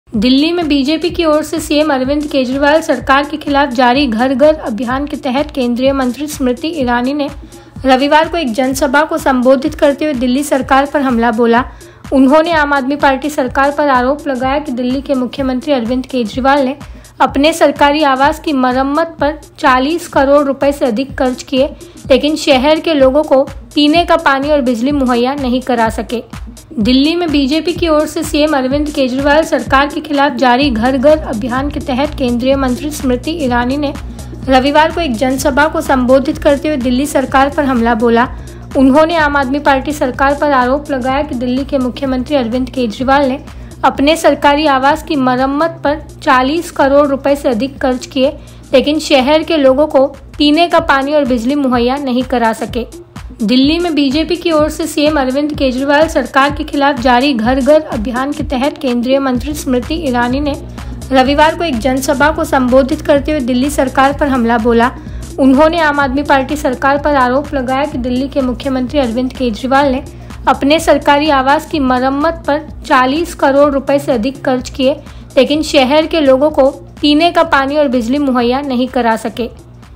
Smriti Irani का दिल्ली के सीएम अरविंद केजरीवाल पर तंज
दिल्ली में बीजेपी (BJP) की ओर से सीएम अरविंद केजरीवाल सरकार के खिलाफ जारी घर-घर अभियान के तहत केंद्रीय मंत्री स्मृति ईरानी (Smriti Irani) ने रविवार को एक जनसभा को संबोधित करते हुए दिल्ली सरकार पर हमला बोला. उन्होंने आम आदमी पार्टी (AAP) सरकार पर  आरोप लगाया कि दिल्ली के मुख्यमंत्री अरविंद केजरीवाल (Arvind Kejriwal) ने अपने सरकारी आवास की मरम्मत पर 40 करोड़ रुपये से अधिक खर्च किए, लेकिन शहर के लोगों को पीने का पानी और बिजली मुहैया नहीं करा सके.प्रधानमंत्री नरेन्द्र मोदी के नेतृत्व वाली सरकार के नौ साल पूरे होने के मौके पर पश्चिमी दिल्ली में आयोजित कार्यक्रम में उन्होंने आरोप लगाया कि सीएम अरविंद केजरीवाल विकास परियोजनाओं के लिए धन नहीं देते हैं, लेकिन श्रेय लेने में सबसे आगे रहते हैं. यह शर्मनाक है कि दिल्ली के मुख्यमंत्री अरविंद केजरीवाल ने सरकारी आवास के निर्माण पर 40 करोड़ रुपये से अधिक खर्च किए, लेकिन राजधानी के लोगों को स्वच्छ पेयजल और बिजली मुहैया नहीं करा सके. ये हाल उस समय है जब वो पिछले नौ साल हर चुनाव में दिल्ली के लोगों से इस बात का वादा करते आये हैं कि 24 घंटे बिजली मुहैया कराएंगे.